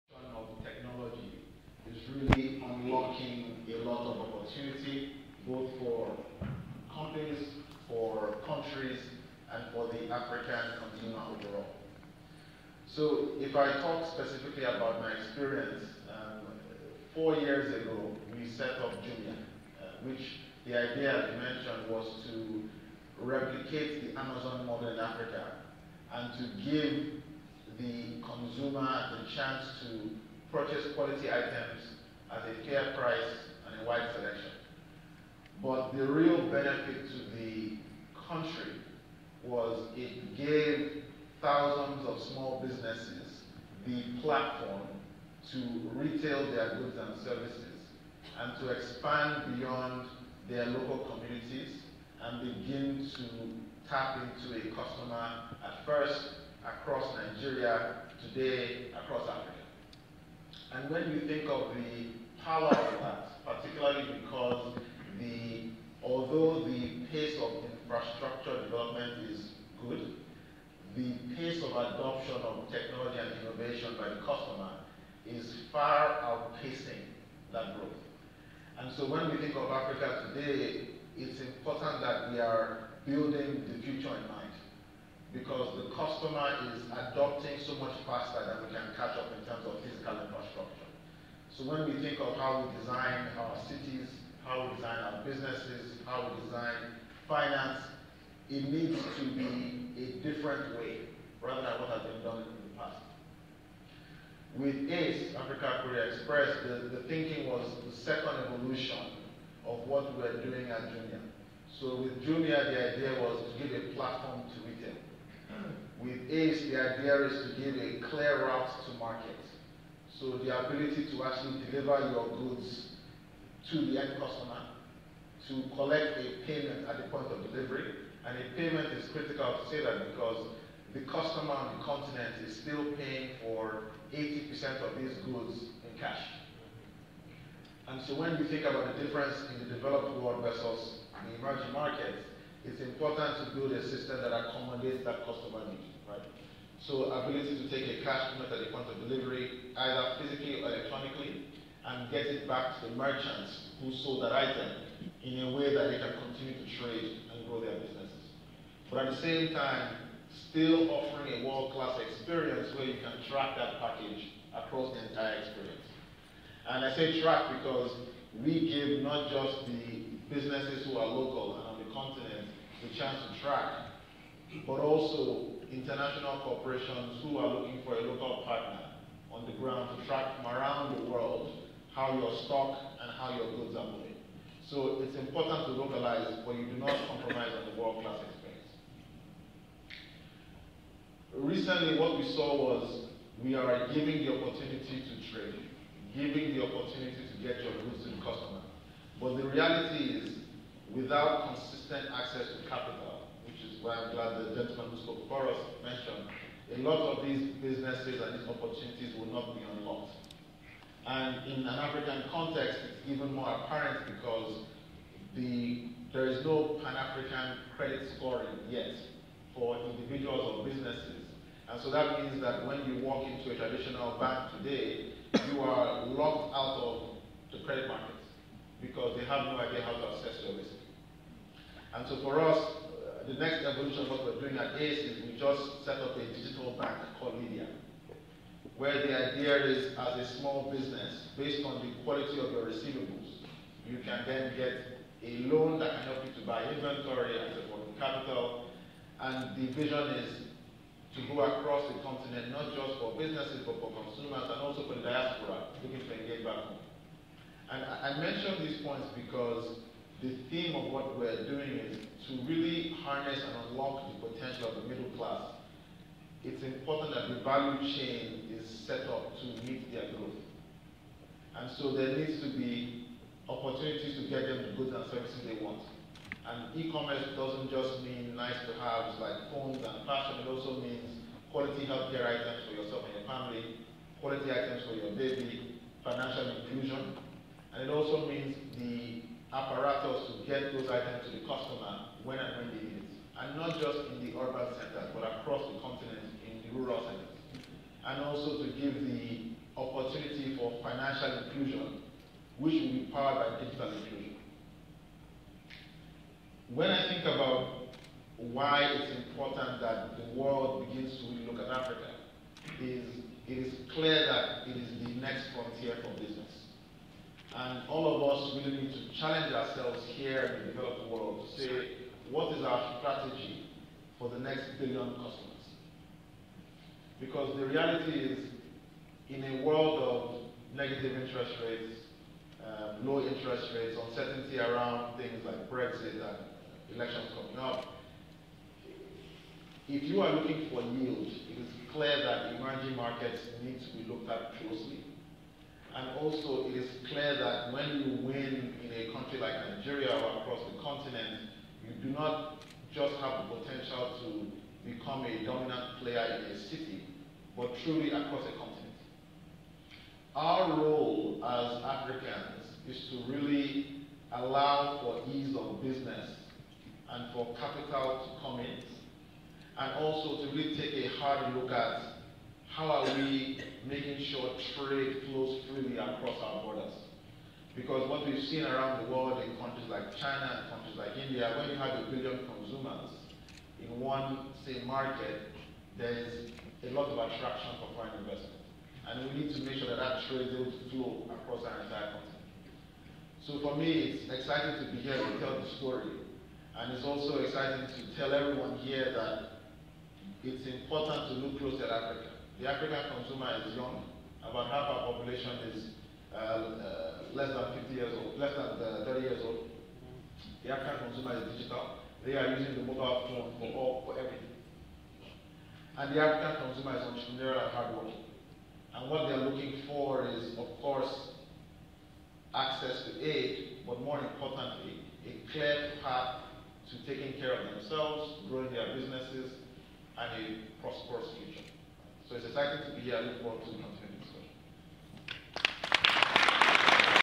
Tunde Kehinde, Africa Courier Express Presentation at TGF
Tunde Kehinde is the Co-Founder and Co-Managing Director of ACE a technology-enabled logistics platform, enabling eCommerce companies, financial institutions and Healthcare businesses to ship items directly to consumers across Nigeria with plans to expand across Africa. ACE shipped to close to 400,000 customers, and collected close to $10 million United States Dollars in payments on behalf of 1,000 merchants.

Tunde Kehinde is the Co-Founder and former Managing Director of Jumia.com, Nigeria’s Number eCommerce Platform which he helped grow the business from 5 employees to close to 1,000 employees serving customers all over Nigeria.

Prior to Jumia, Tunde co-founded Bandeka.com, an online dating platform identified by Forbes as one of the hottest tech start-ups in Africa. Tunde has prior experience as a business development executive with Diageo in London and as an Investment Banking professional with Wachovia Securities in North Carolina and New York City.

Tunde holds a degree in Finance with honors from Howard University and an MBA Harvard Business School.